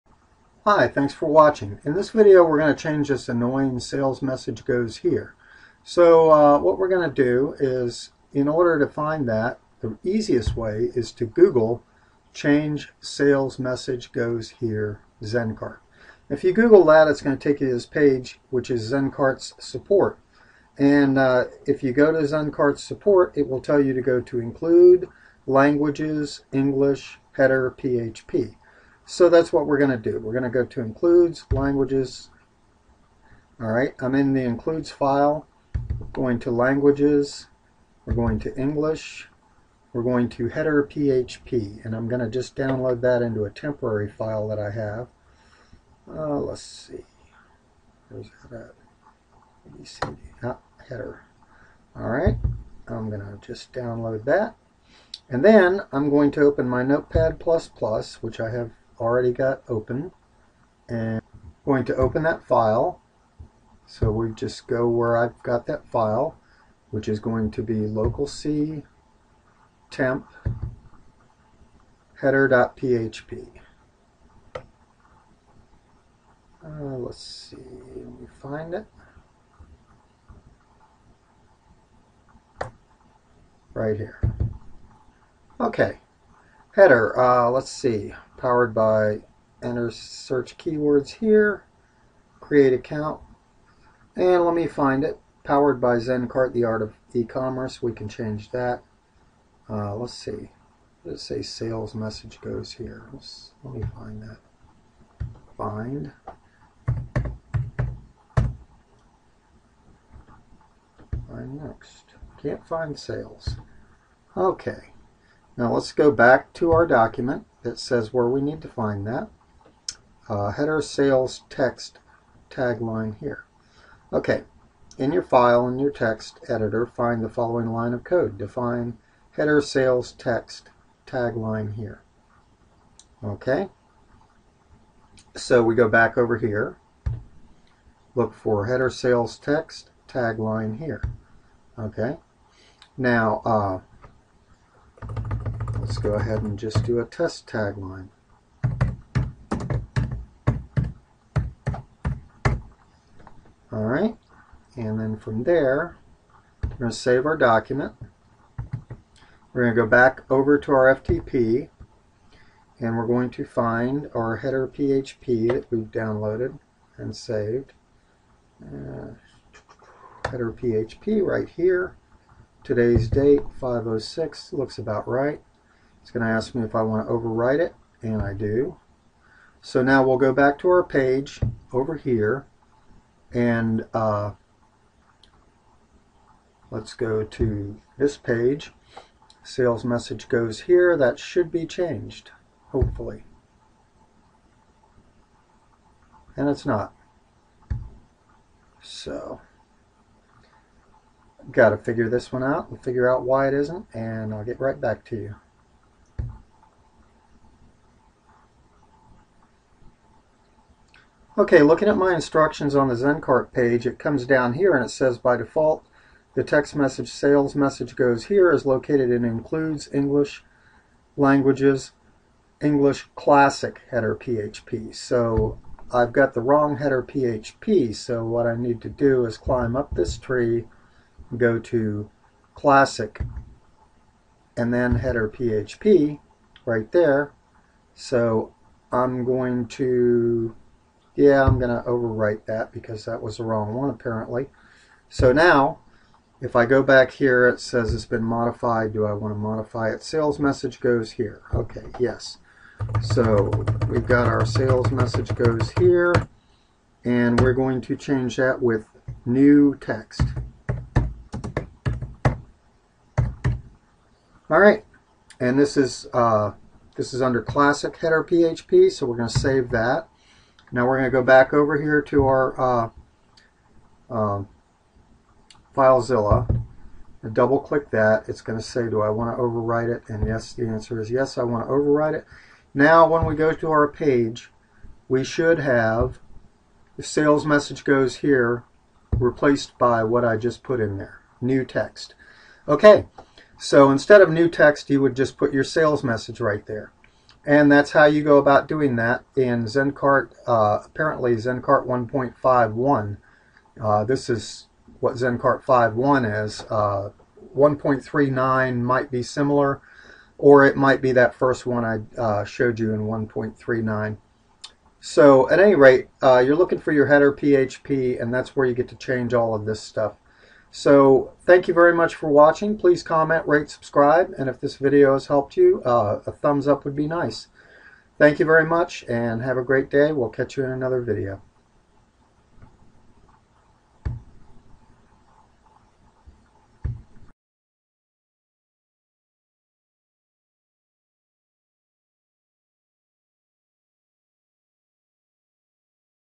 Zen Cart Remove "Sales Message Goes Here" when Installing ZenCart
The Zencart  "Sales Message Goes Here" is hard coded in a php document. I wish some of these things were done in the database but for now you have to manually edit the file. Here is how to find these instructions plus a failed, then successful edit. (The instructions seem to be updated for version 1.5.1)